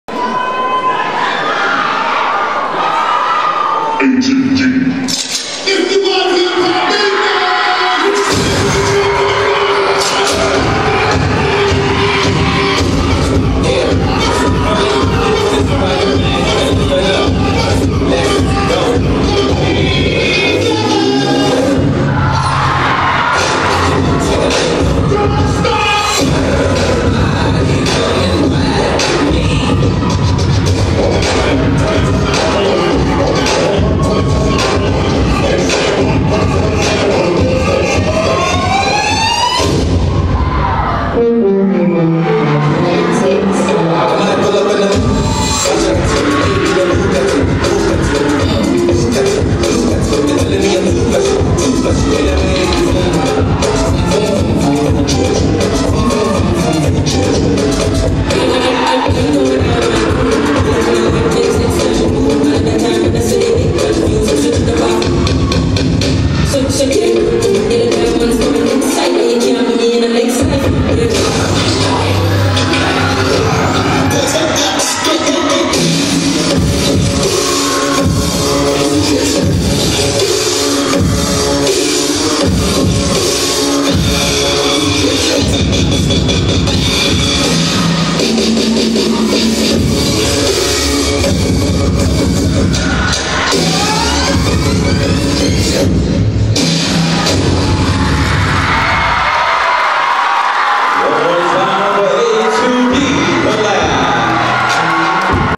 A2D Co-Lab - Australia | MiniCrew Division | 2024 World Hip Hop Dance Championship Prelims
The world's best dance crews traveled to Phoenix, AZ, to compete for Gold, Silver, Bronze, and the title of World Champion at the 2024 World Hip Hop Dance Championship.

The best advanced to the finals, which were held at the Mullett Arena on the campus of Arizona State University.